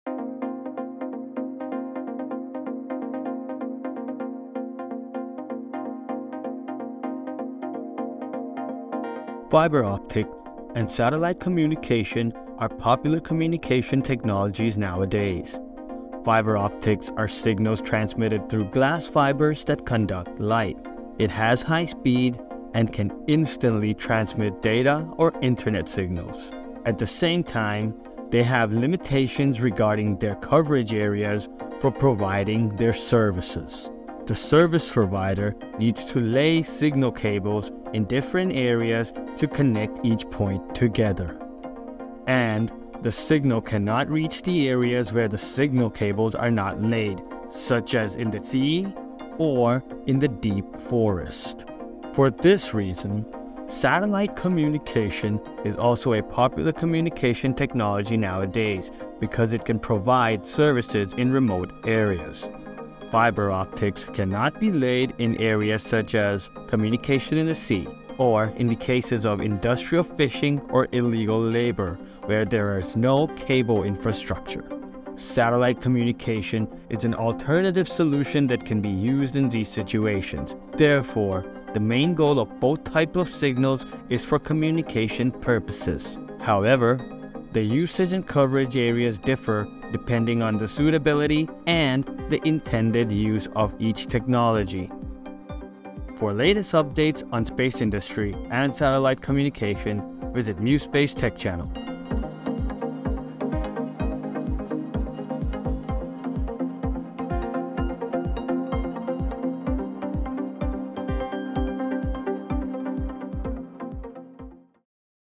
Fiber Optical VS Satellite Communication | mu Space
When the world already has internet connectivity through FiberOptical cables, why is SatelliteInternet still necessary for many industries?

What are the differences between these two types of internet?
Find the answers in this video!

ในเมื่อเรามี internet ที่ได้จากการลากสายสัญญาของ Fiber Optic อยู่แล้ว… “ทำไม Satellite Internet ยังจำเป็นสำหรับหลาย ๆ กลุ่มอุตสาหกรรม?”

ทั้งในแง่ของลักษณะการทำงาน และการนำมาใช้งานจริง ความแตกต่างของอินเทอร์เน็ตทั้ง 2 รูปแบบนี้คืออะไร หาคำตอบไปพร้อมกันได้ในวิดีโอนี้!